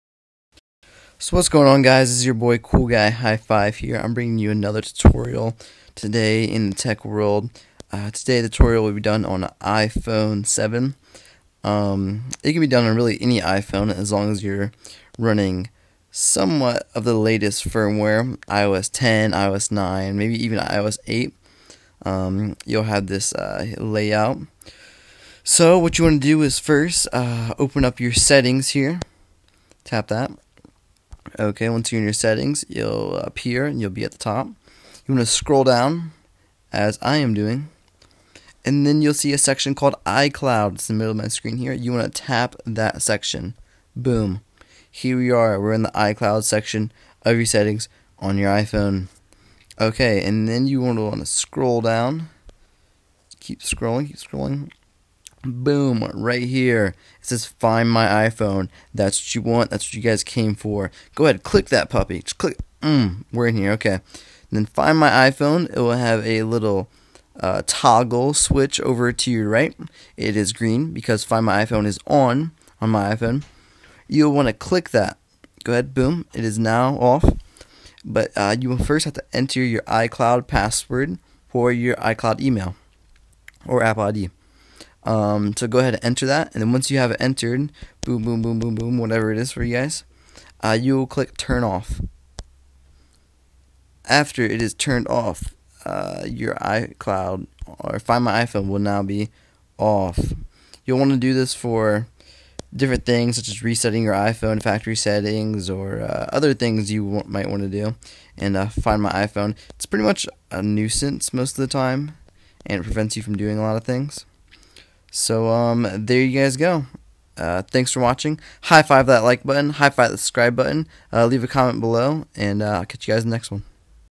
How to turn off find my iPhone!!! (iPhone 7, 6, 6s, 5, 5s) working 2017!!!
This tutorial will be showing you guys how to turn off find my iPhone in any iPhone!!! Working 2017!!! Any iOS!!! High five that like button!!